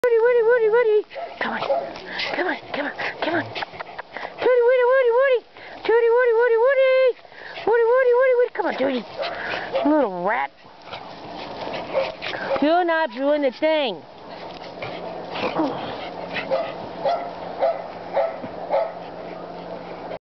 Corgi dances
Tuddy dances to her name.